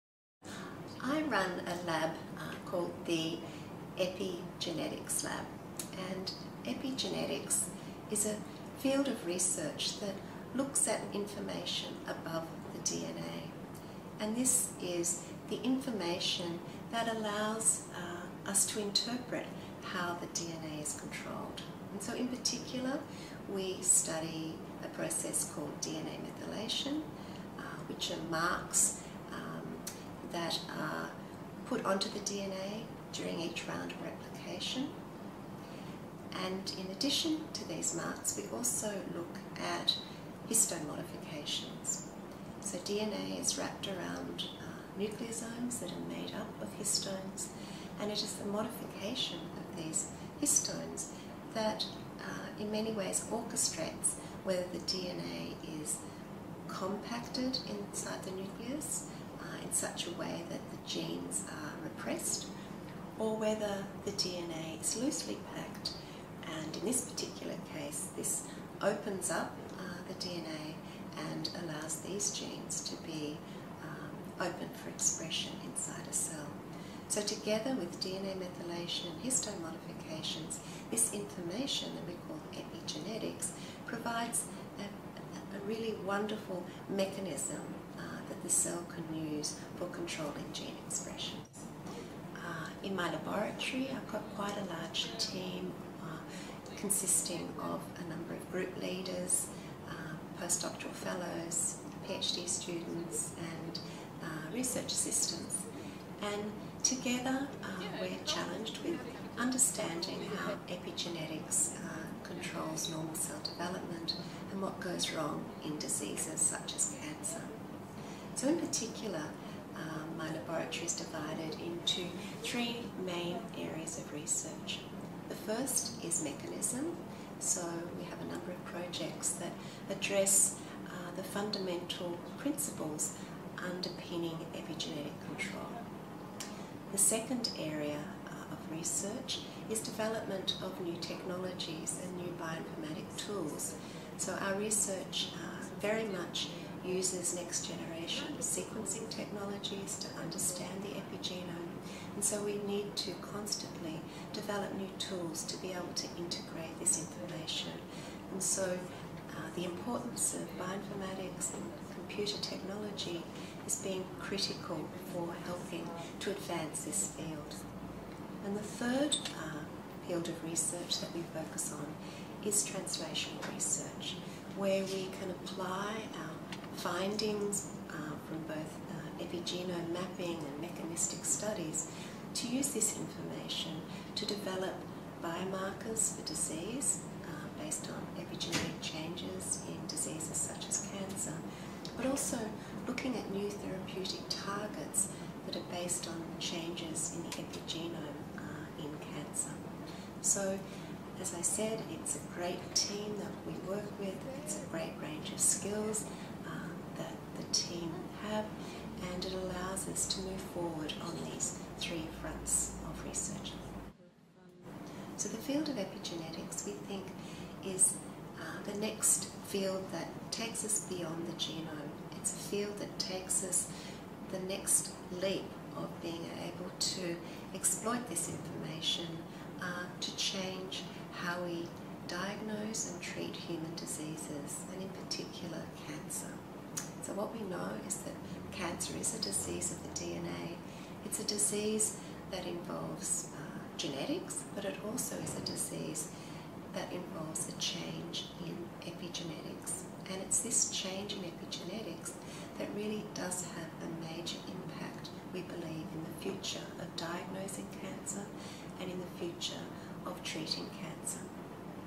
Professor Susan Clark
Professor Susan Clark is Head of the Genomics and Epigenetics Division at the Garvan Institute of Medical Research in Sydney